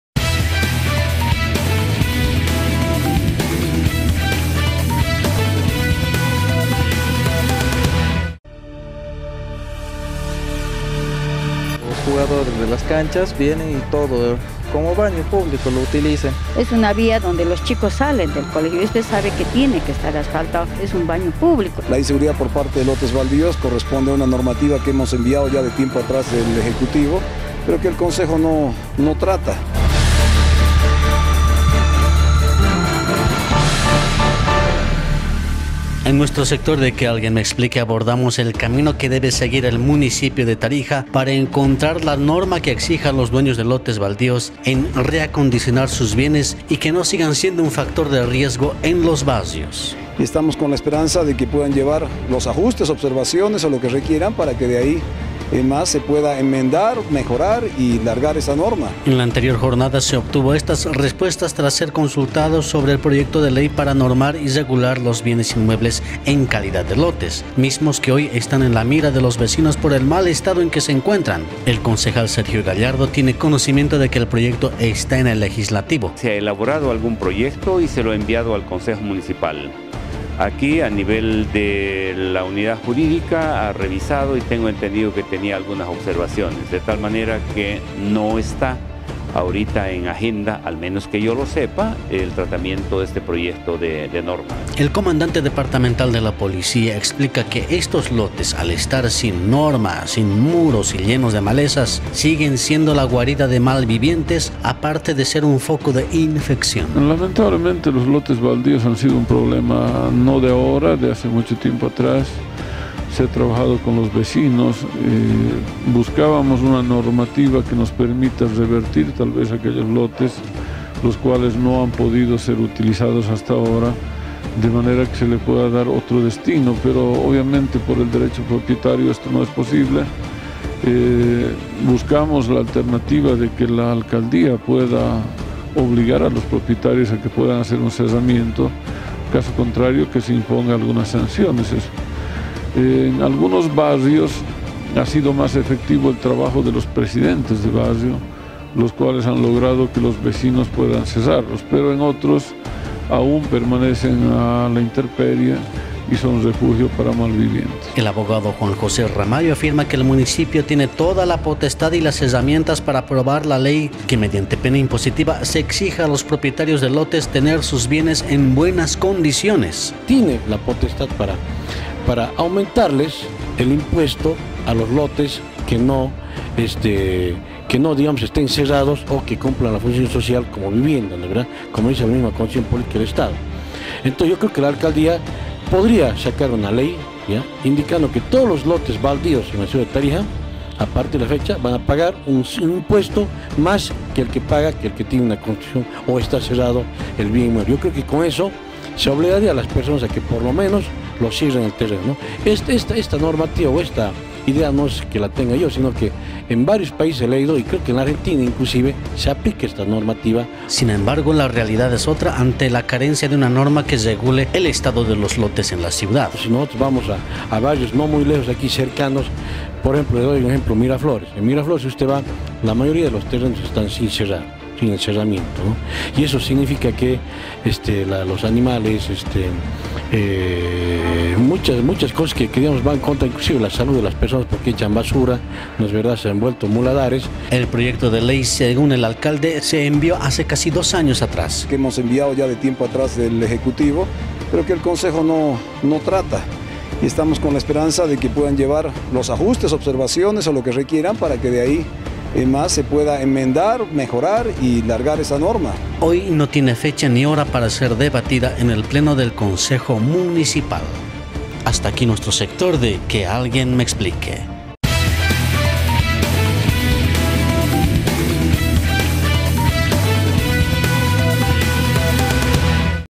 QUE ALGUIEN ME EXPLIQUE: NORMA IMPOSITIVA PARA REGULAR LOTES BALDÍOS
Proyecto de ley para normar y regular los bienes inmuebles en calidad de lotes.